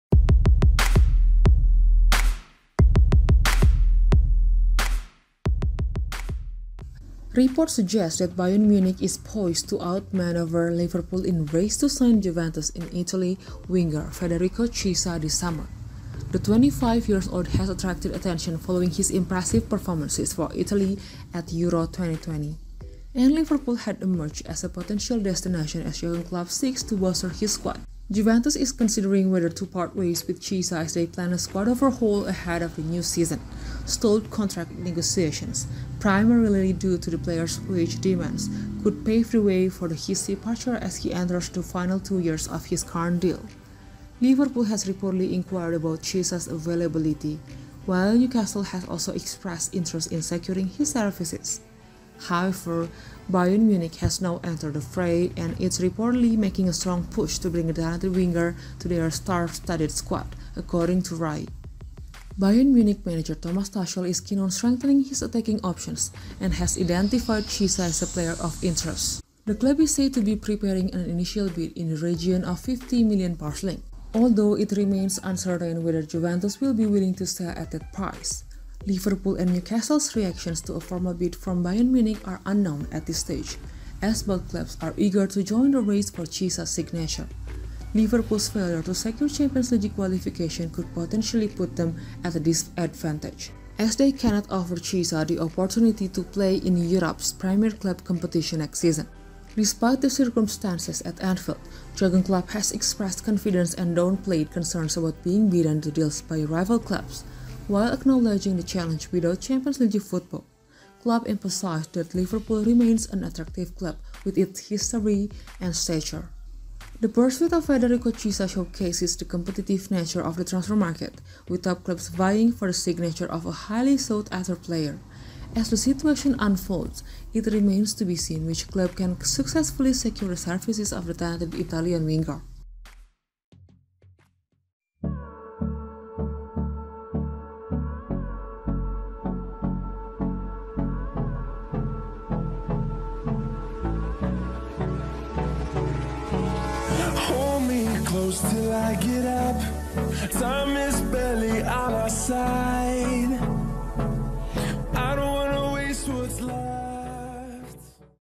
LIVERPOOL SHOCKED AS BAYERN MUNICH HIJACKS CHIESA DEAL: Transfer Showdown Unveiled!
Incredible transfer twist! Liverpool FC left stunned as Bayern Munich outmaneuvers them in the race for Federico Chiesa's signature! Get the latest scoop on Liverpool's pursuit of the Italian winger and the intense competition from Newcastle and now Bayern Munich. Will Liverpool's lack of Champions League football prove to be a disadvantage? Discover the staggering bid Bayern Munich is preparing and Juventus' decision on selling Chiesa. Don't miss this thrilling transfer showdown as top clubs battle for one of Europe's hottest talents!